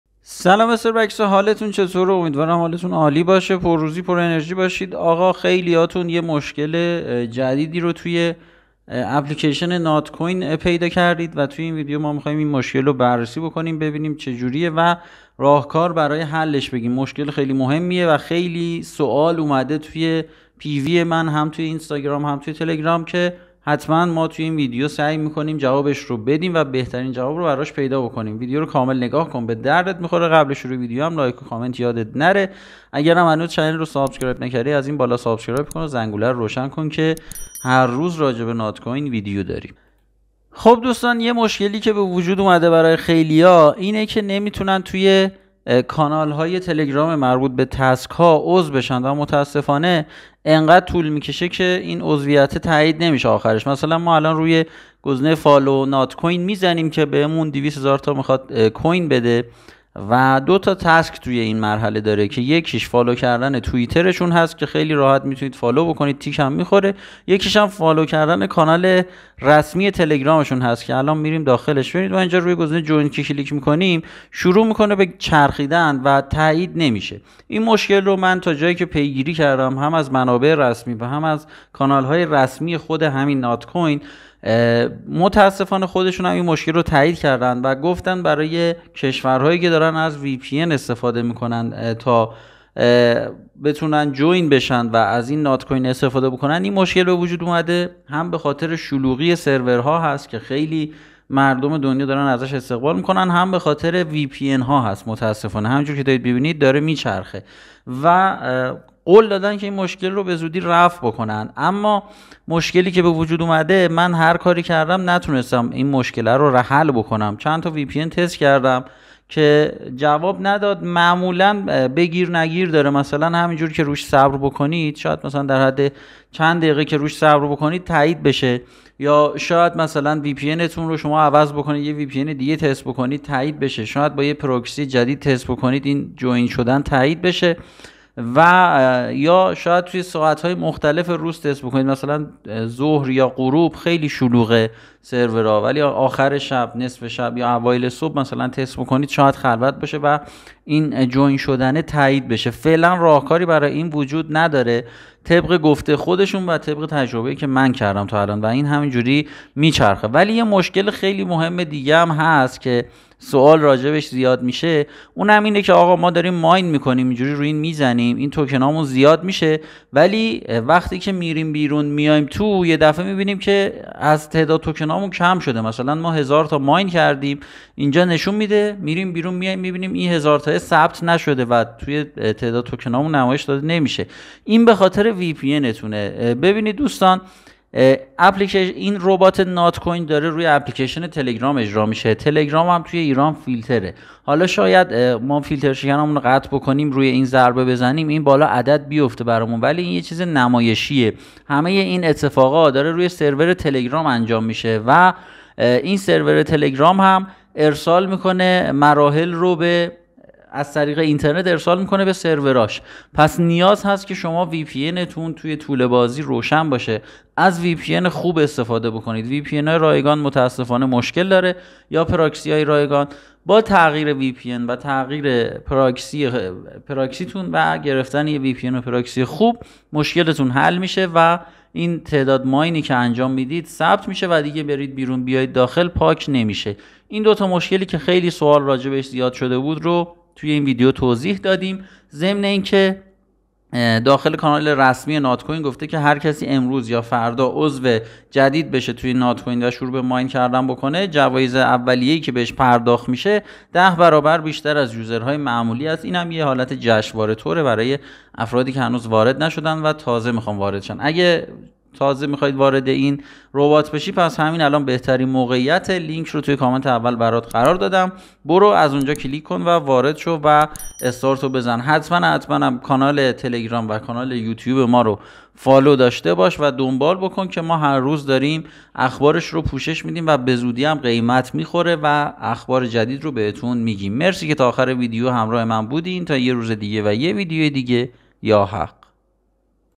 سکه هام تو نات کوین کم میشه هی !! چیکار کنم ؟؟ ❌ با این ترفند مشکلتو حل کن ❌
اگه شما هم تو نات کوین مشکلی دارید با کم شدن سکه هاتون و هربار میرید بیرون و میاید داخل میبینید سکه هاتون کم شده یا اگه نمیتونید داخل کانال های تلگرامیش جوین بشید این ویدیو رو نگاه کنید که به دردتون میخوره و مشکلتون رو حل میکنه ...
لطفا جهت حمایت ازما کانال رو سابسکرایب کنید✅
و کلیپ رو لایک کنید🌺